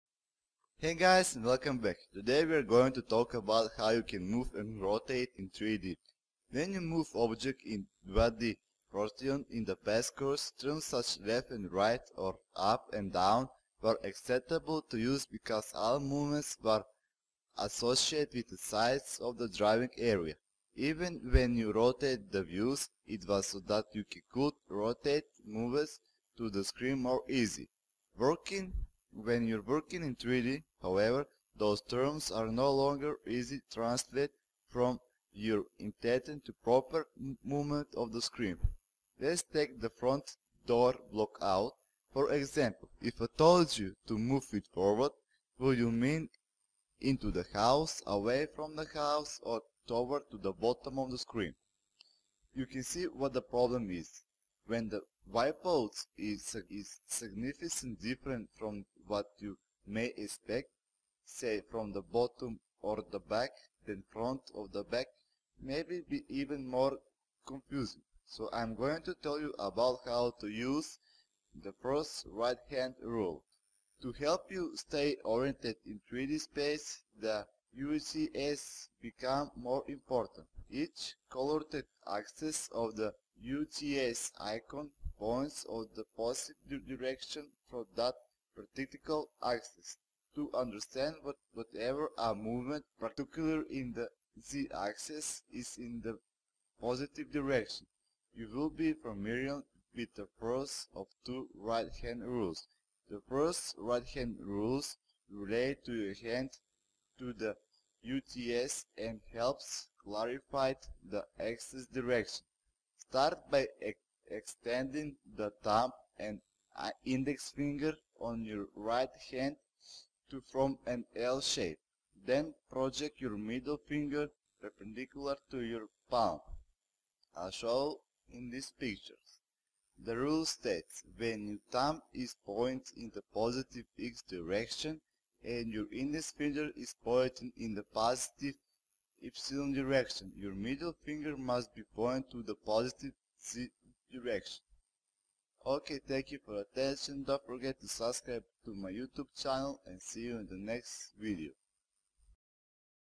Autocad 2016 3D Modeling: Moving and Rotate in 3D Lesson 6
These lessons will give you introduction to the AutoCAD interface, the most used tools and how to access them and reduce the initial feeling of software tool overload that is very common with beginners as you progress into the more advanced territory.